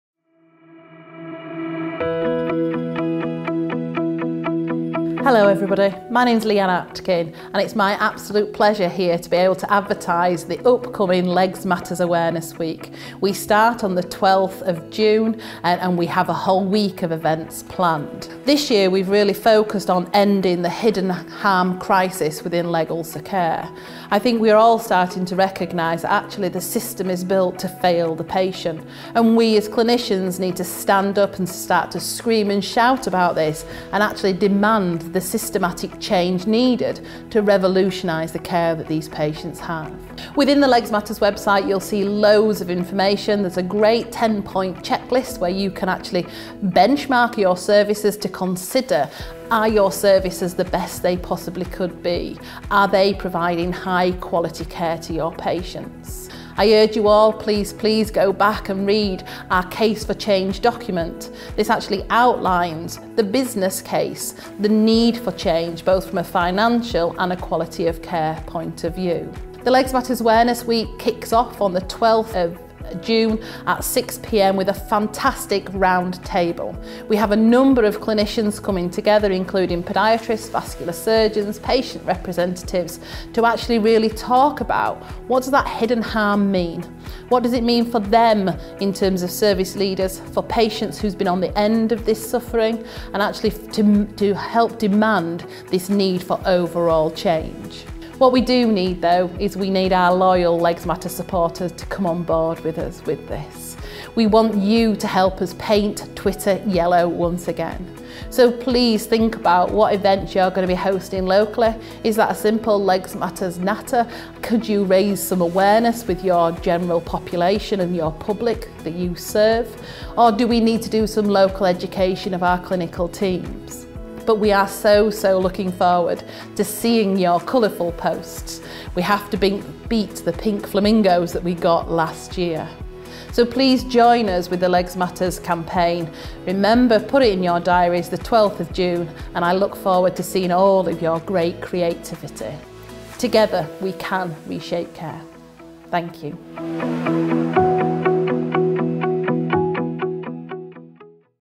Legs Matter Awareness Week 2023 - with Dr Leanne Atkin
Listen to Leanne talk about whats happening during the week and how the Legs Matter Campaign is shining a light on the hidden harm crisis in leg and foot care.